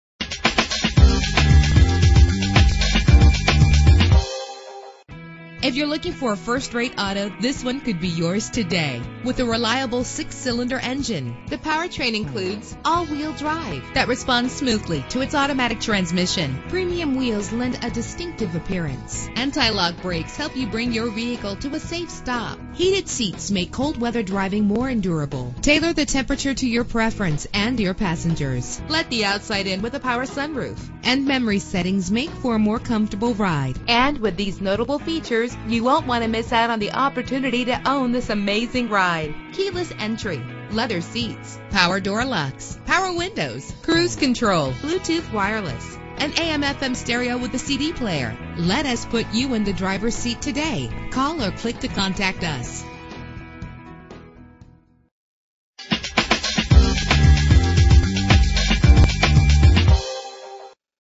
2012 Audi A7 - Atlanta GA
Year: 2012
Make: Audi
Model: A7
Trim: 3.0T quattro Premium
Engine: 3.0 liter 6 cylinder 24 valve
Transmission: 8-Speed Automatic
Color: Quartz Gray Metallic
Mileage: 26630
Address:
5901 Peachtree Industrial Blvd